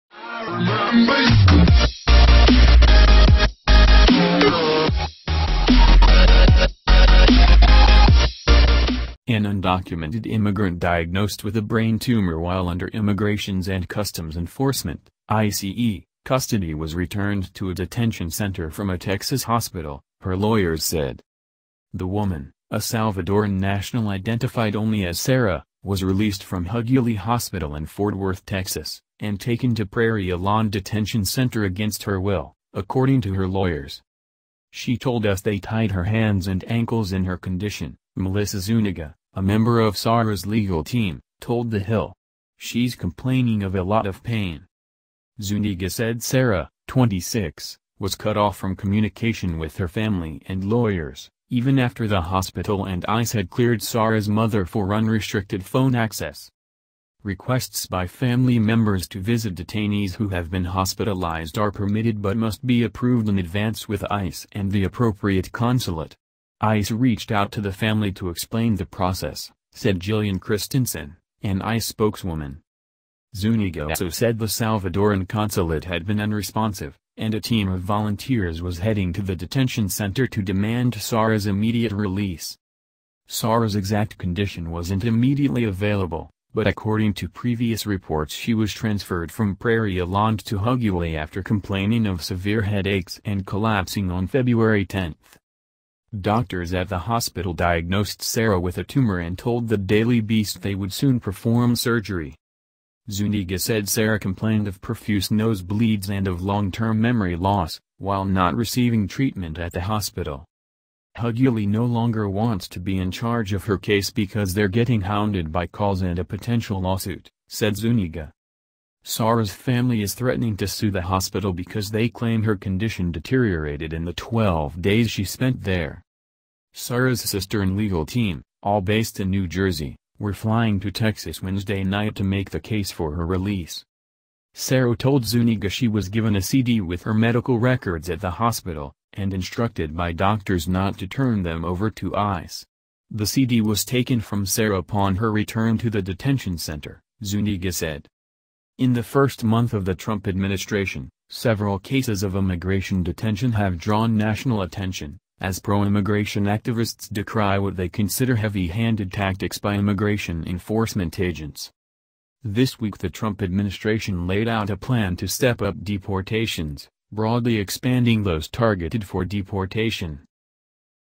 Lawyers: ICE detainee with brain tumor removed from hospital
An undocumented immigrant diagnosed with a brain tumor while under Immigrations and Customs Enforcement (ICE) custody was returned to a detention center from a Texas hospital, her lawyers said.